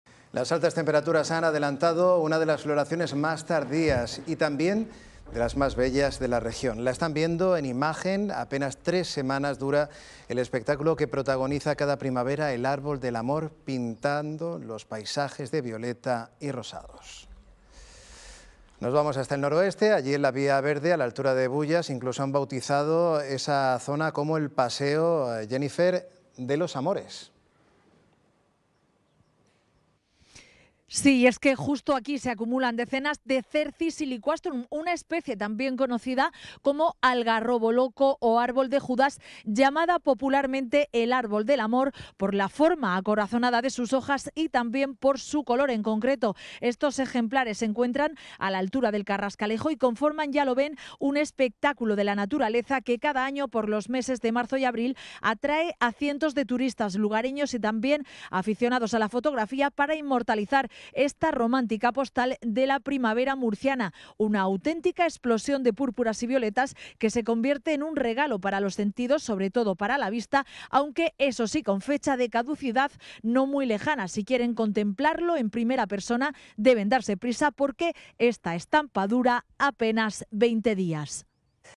La floración del árbol del amor del Noroeste se adelanta por las altas temperaturas | La 7
Las altas temperaturas han adelantado una de las floraciones más tardías y bellas en la Región de Murcia. Se trata de la del árbol del amor, a la altura de la Vía Verde del Noroeste, que apenas dura veinte días.